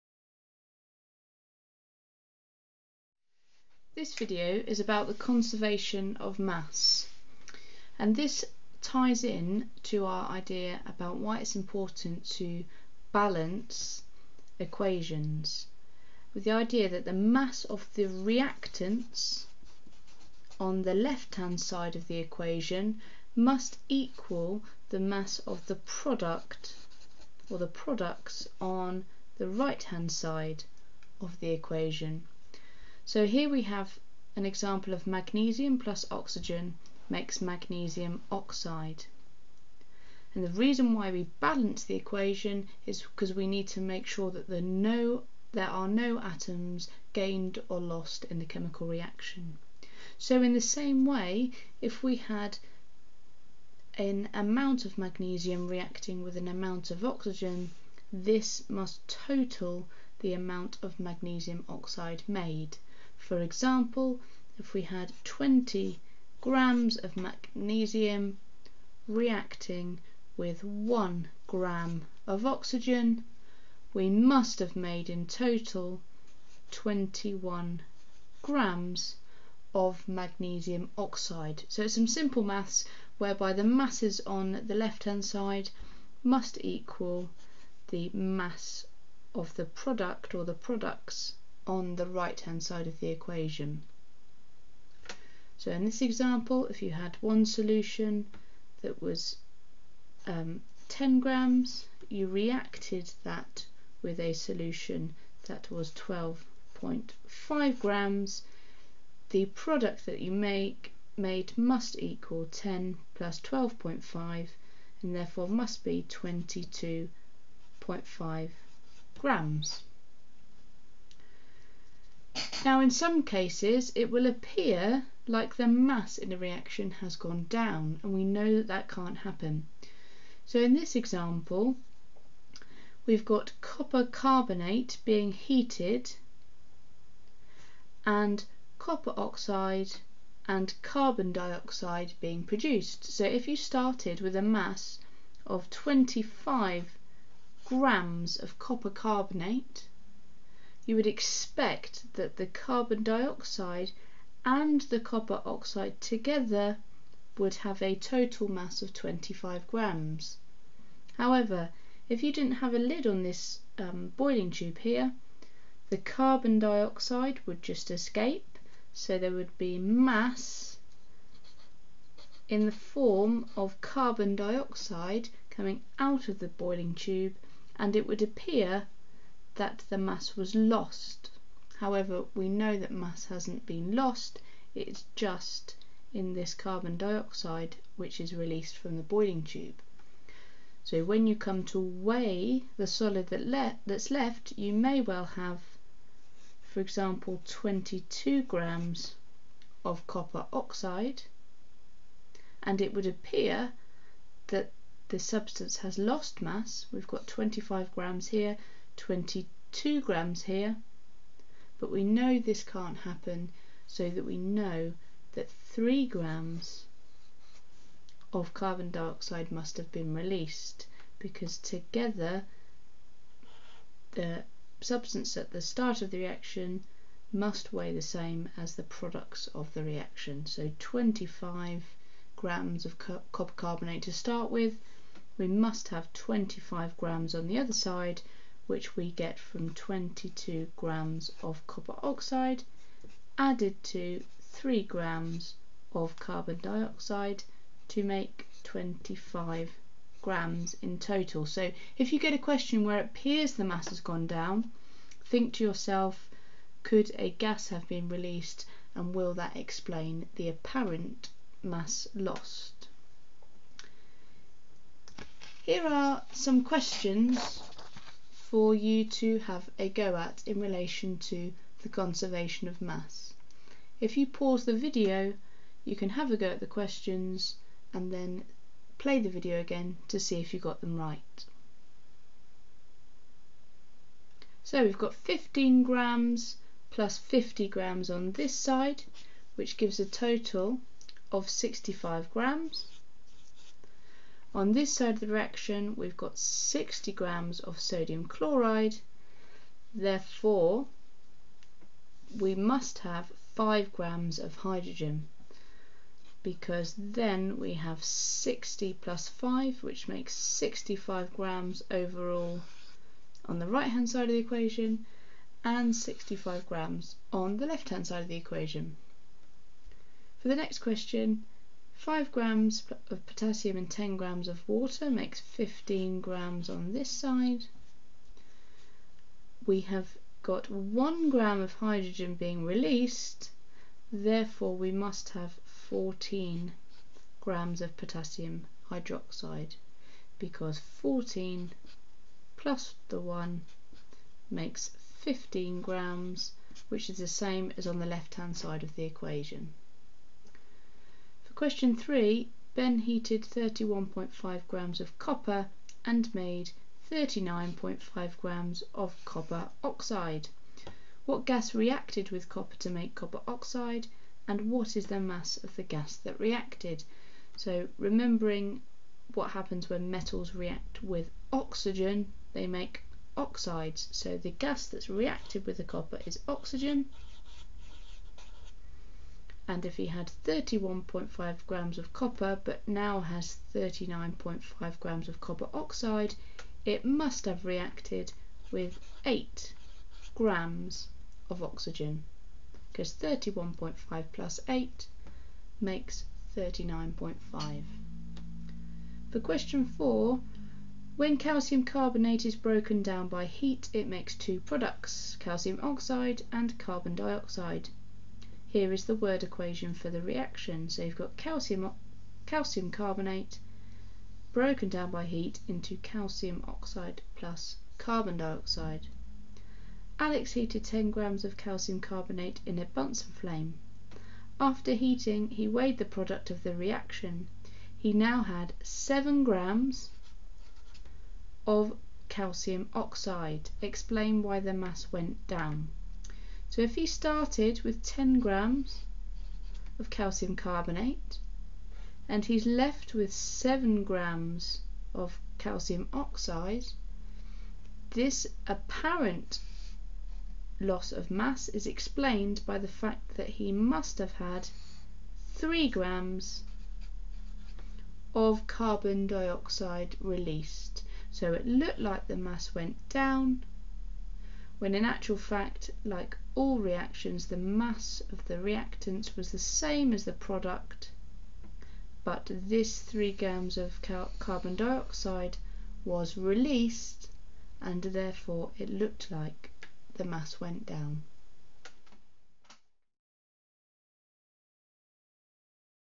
KS3 - Conservation of Mass
This video is about Conservation of Mass. This video is for Key Stage Three pupils studying Science, however it is also useful for Key Stage Four Pupils.